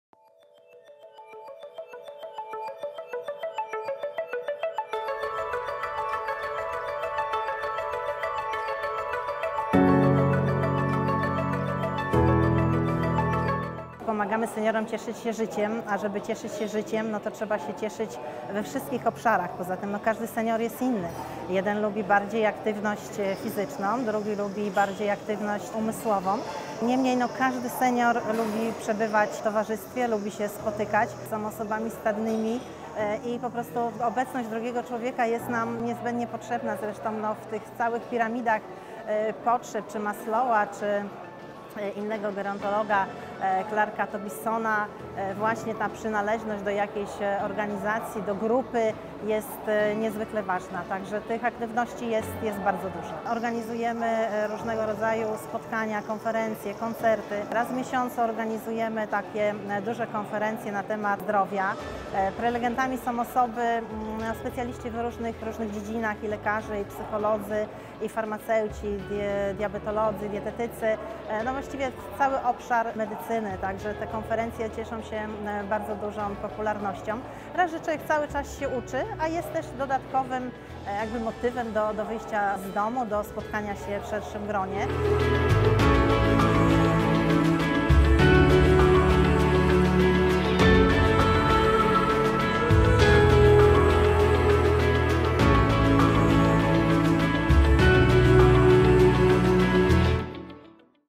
„Pacjent w centrum uwagi” - nie tylko postulat, ale i konieczność
Trend demograficzny jest jasny - jako społeczeństwo będziemy się starzeć, co wymagać będzie zarówno zapewnienia seniorom odpowiedniej infrastruktury czy opieki, jak i zmiany ich statusu w społeczeństwie. „Osoby starsze, podobnie jak ludzie w innych kategoriach wiekowych, mają takie same potrzeby: aktywnego spędzania czasu, spotykania bliskich i przyjaciół, angażowania się w kolejne inicjatywy. Naszym celem jest stworzenie seniorom przestrzeni, w której będą mogli czuć się swobodnie, widywać się i wymieniać doświadczeniami, wspólnie zdobywać nową wiedzę i umiejętności” – przekonywała Elżbieta Łacina, prezeska DOZ Fundacji, podczas III Ogólnopolskiego Kongresu „Pacjent w Centrum Uwagi” zorganizowanego przez DOZ S.A.